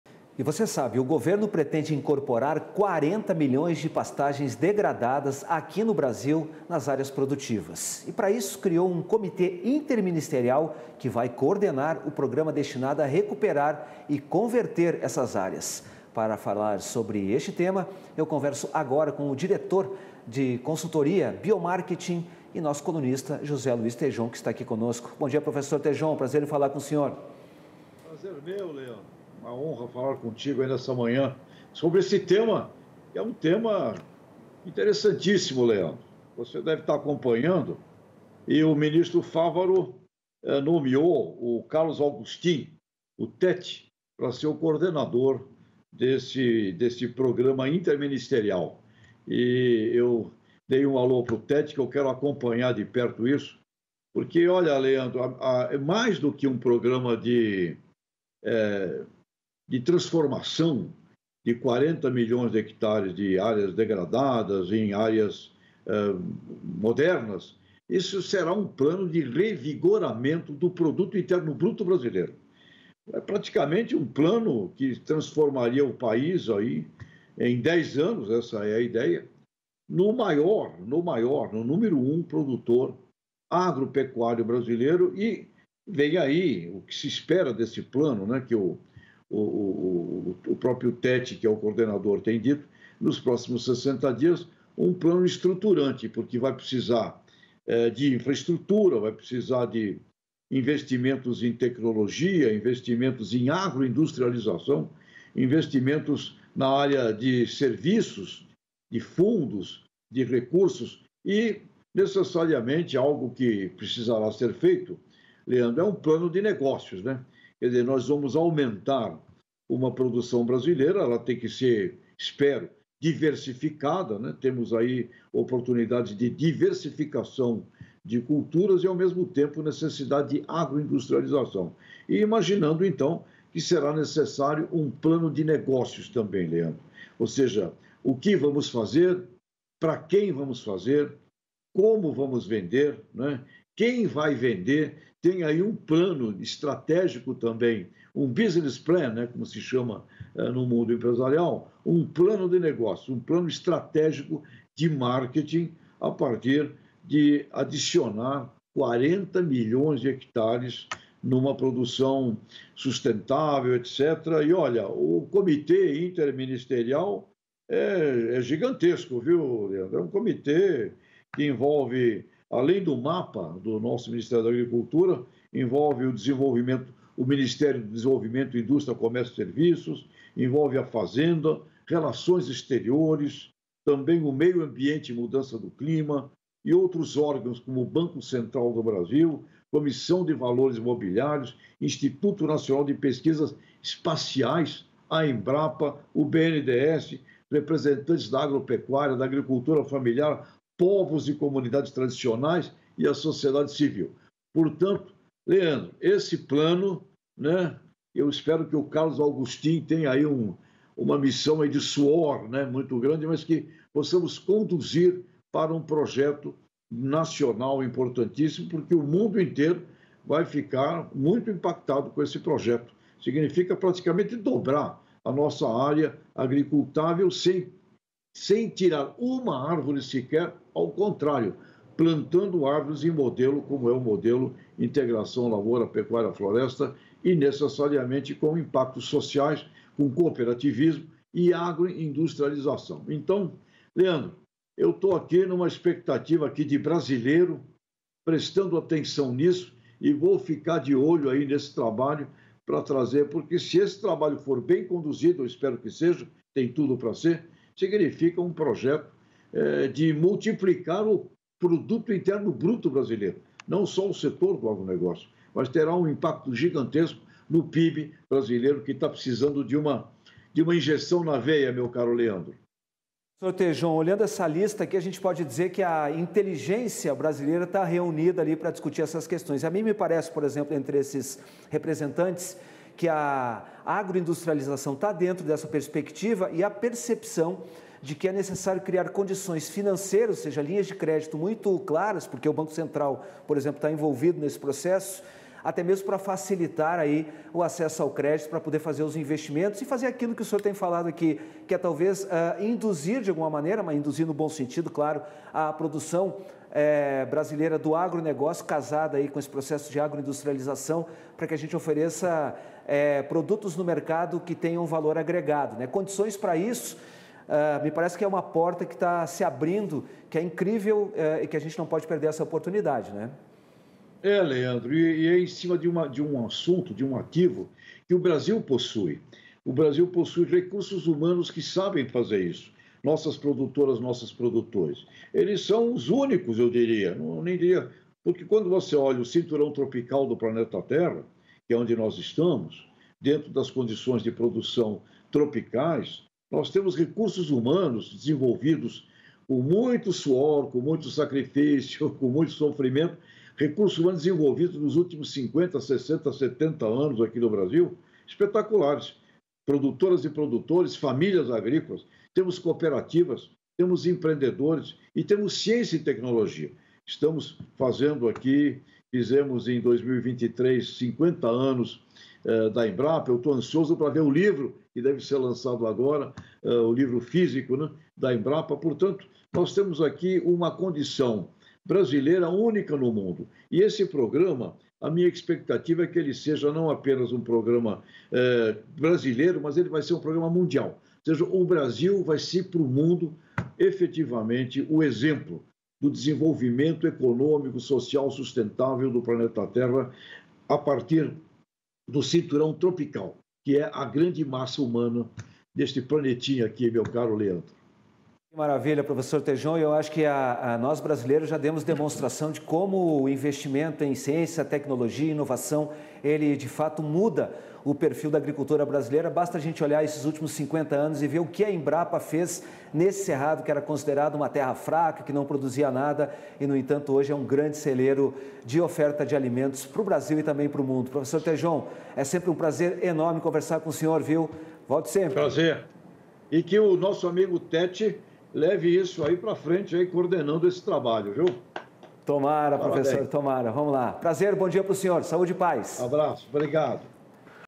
Análise Tejon: programa de recuperação de 40 milhões de hectares
O governo pretende incorporar 40 milhões de hectares de pastagens degradadas aqui no Brasil nas áreas produtivas. Para isso, criou um comitê interministerial que vai coordenar o programa destinado a recuperar e converter essas áreas. Sobre esse tema, confira a análise do diretor da consultoria biomarketing, e nosso colunista, José Luiz Tejon.